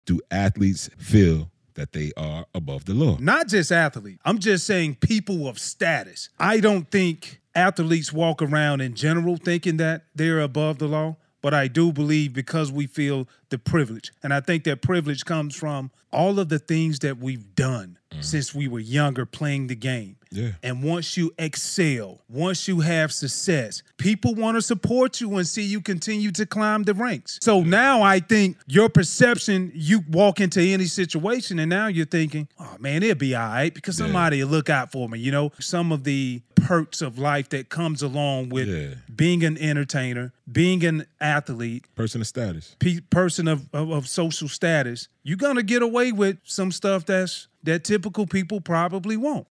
IS ATHLETE PRIVILEGE A STATUS THING, OR IS IT EARNED?
Former NFL players Takeo Spikes & Tutan Reyes discuss this controversial topic.

Co-Host & Executive Producer: Tutan Reyes
Produced by Behind the Mask Media, LLC
Edited and YouTube Management by  @Devin Dismang
Videography: Simone Kilgore
Audio Engineer: Anthony Majors